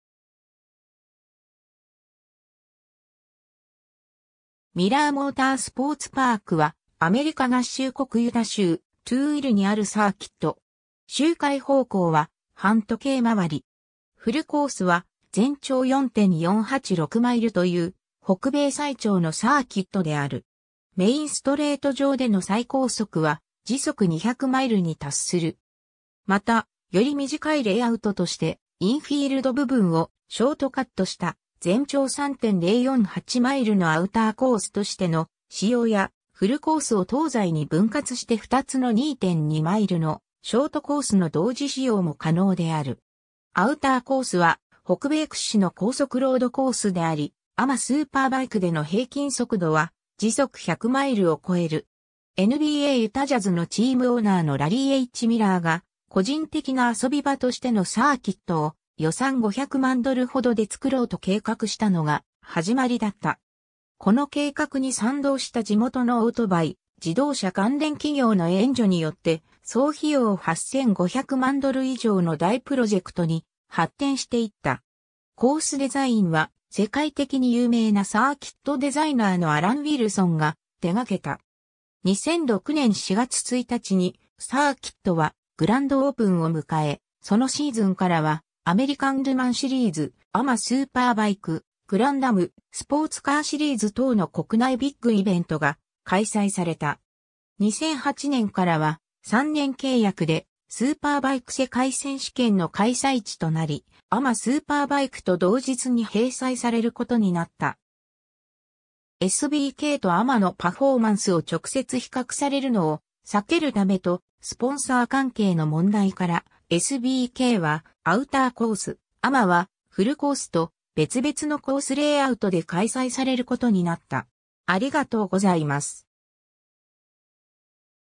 ミラー・モータースポーツ・パーク
ミラー・モータースポーツパーク (Miller Motorsports Park) は、アメリカ合衆国ユタ州トゥーイルにあるサーキット。
 周回方向は反時計回り。
フルコースは全長4.486マイル(7.220km)という、北米最長のサーキットである。
メインストレート上での最高速は時速200マイル(320km/h)に達する。
また、より短いレイアウトとして、インフィールド部分をショートカットした全長3.048 マイル(4.905 km)のアウターコースとしての使用や、フルコースを東西に分割して2つの2.2マイル(3.5 km) のショートコースの同時使用も可能である。
アウターコースは北米屈指の高速ロードコースであり、AMAスーパーバイクでの平均速度は時速100マイル(160km/h)を超える。
 NBAユタ・ジャズのチームオーナーのラリー・H・ミラーが、個人的な遊び場としてのサーキットを予算5百万ドルほどで作ろうと計画したのが始まりだった。
この計画に賛同した地元のオートバイ・自動車関連企業の援助によって、総費用8千5百万ドル以上の大プロジェクトに発展していった。
 コースデザインは、世界的に有名なサーキットデザイナーのアラン・ウィルソンが手がけた。
2006年4月1日にサーキットはグランドオープンを迎え、そのシーズンからはアメリカン・ル・マン・シリーズ、AMAスーパーバイク、グランダム・スポーツカー・シリーズ等の国内ビッグイベントが開催された。
 2008年からは、3年契約でスーパーバイク世界選手権(SBK)の開催地となり、AMAスーパーバイクと同日に併催されることになった。
SBKとAMAのパフォーマンスを直接比較されるのを避けるためと、スポンサー関係の問題から、SBKはアウターコース、AMAはフルコースと、別々のコースレイアウトで開催されることになった。